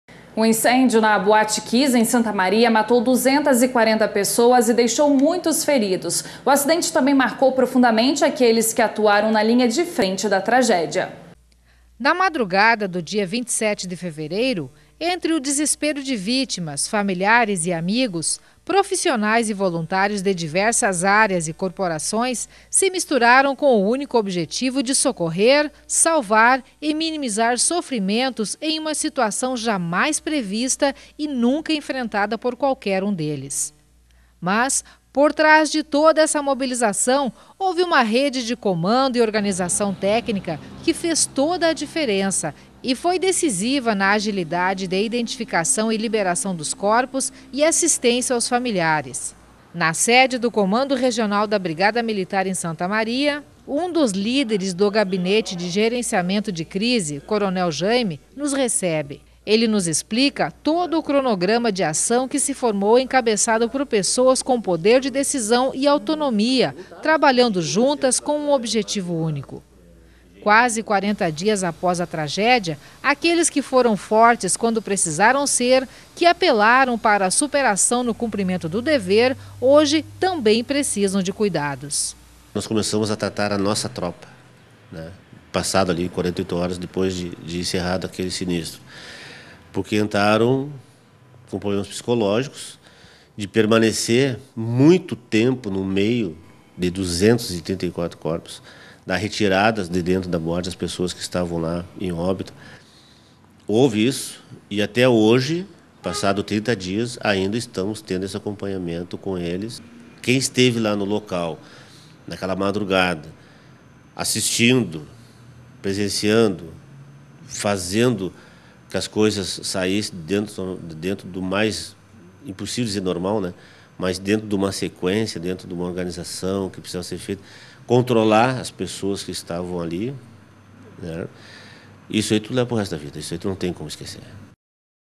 Santa Maria brigada
O incêndio na boate Kiss, em Santa Maria, matou  240 pessoas e deixou muitos feridos. O acidente  também marcou  profundamente aqueles que atuaram na linha de frente da tragédia.